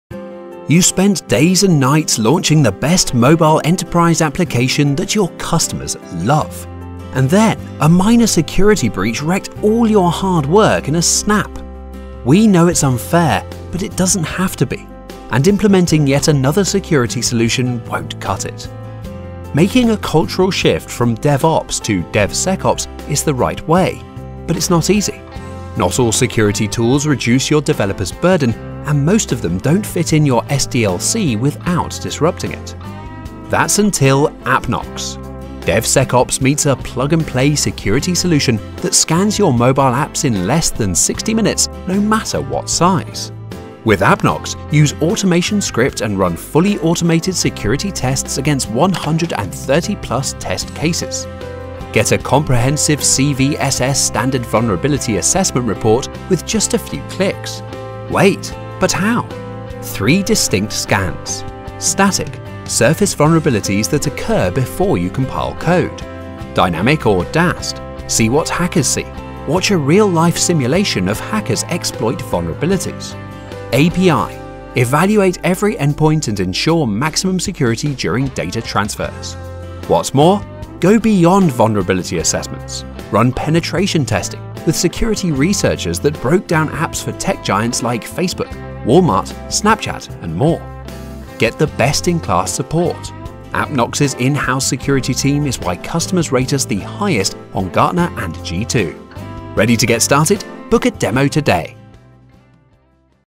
Appknox - Highest Rated Mobile Application Security Solution
Launching a mobile enterprise application is no easy feat and one minor security breach can undo all your hard work in no time. With the right security platform, you can detect and fix security vulnerabilities without losing sleep.

Say hello to Appknox, a plug-and-play security solution that secures your mobile enterprise applications in less than 60 minutes.

Rated the highest in security products in Gartner and being a high performer on G2crowd for SAST, we set ourselves apart from our competition by allowing you to integrate your SDLC with all project management and CI-CD toolchain.

With Appknox, you can reduce delivery timelines and manpower costs and mitigate security threats and get ahead of the competition.